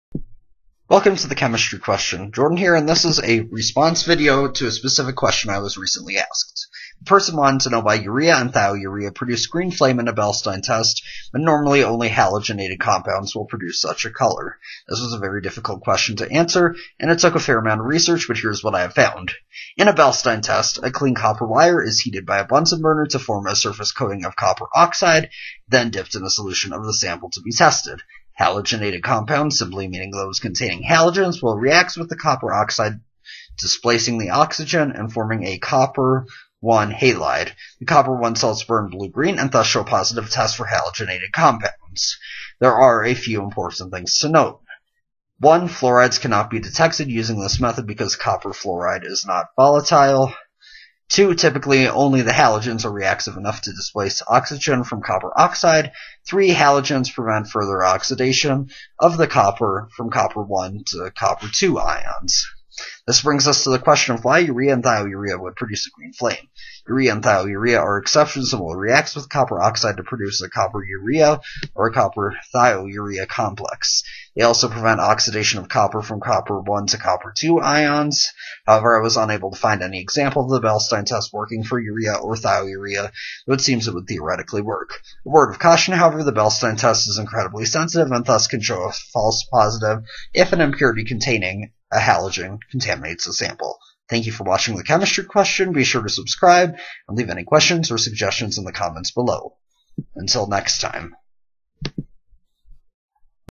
Urea, Thiourea, and the Beilstein Test
This is a response to a specific question I was asked.  Took me two days worth of research, during which time I consulted with three chemistry professors in addition to the internet.  Here is what I found.